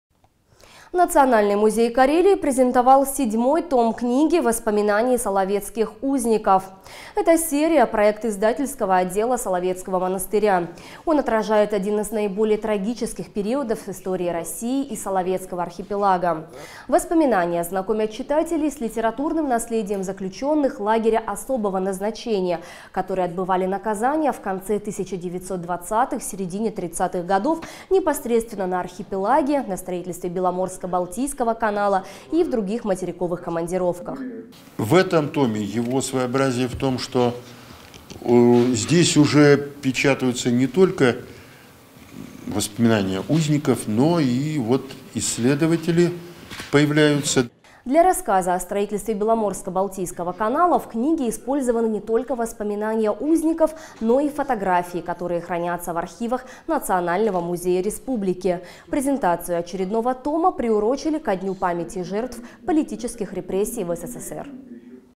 «Воспоминания соловецких узников»
Национальный музей Карелии презентовал седьмой том книги «Воспоминаний соловецких узников». Эта серия — проект издательского отдела Соловецкого монастыря. Он отражает один из наиболее трагических периодов в истории России и Соловецкого архипелага.